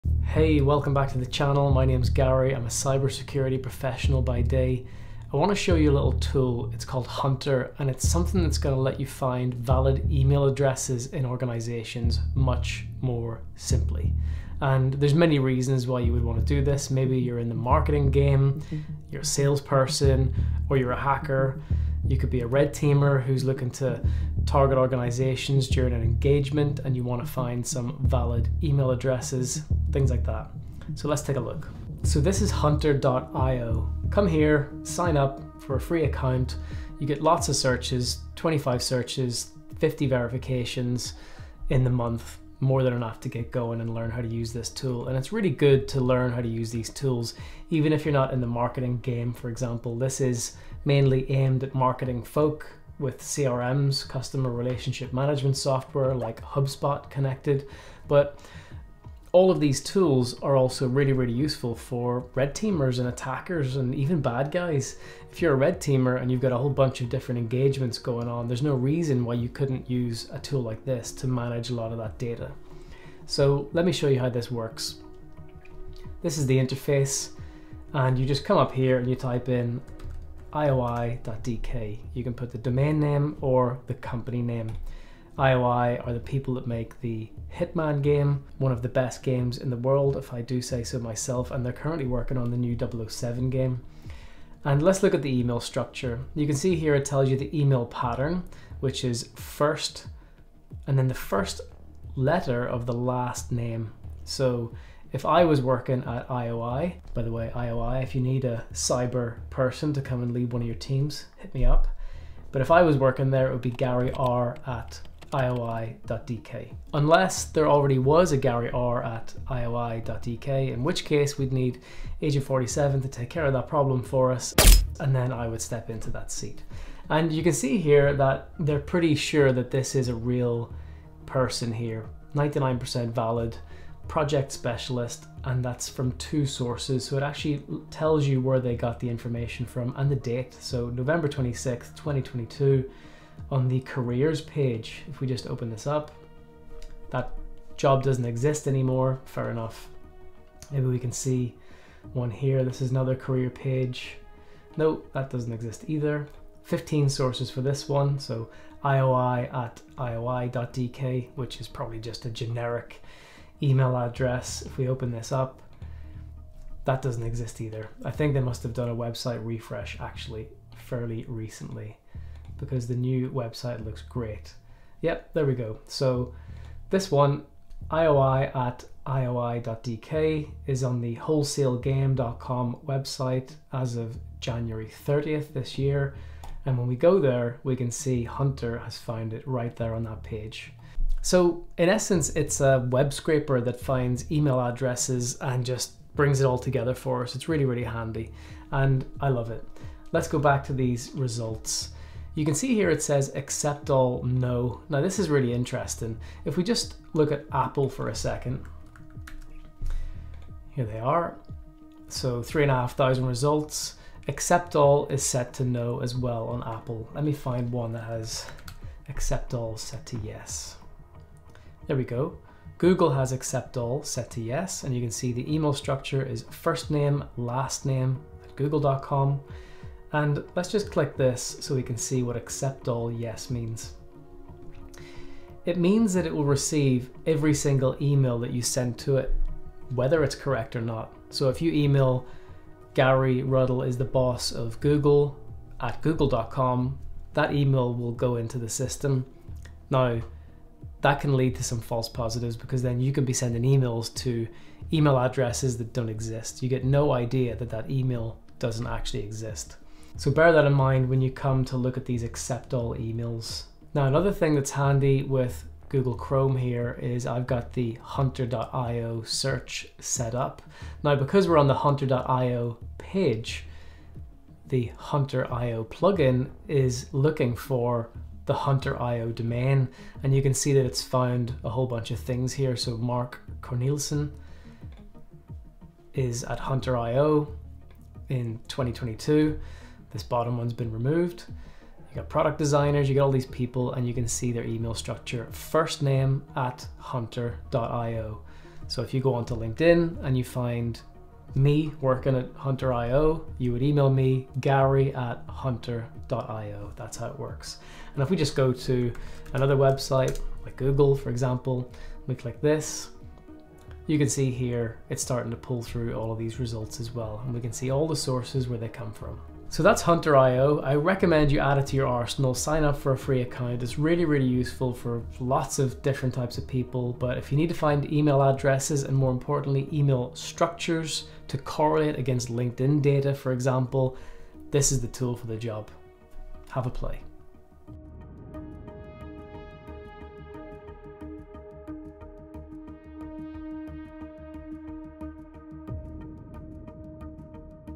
OSINT Skills: Find valid email addresses FAST!
In this tutorial, I'll show you how to find email addresses for use in marketing operations or ethical hacking expeditions!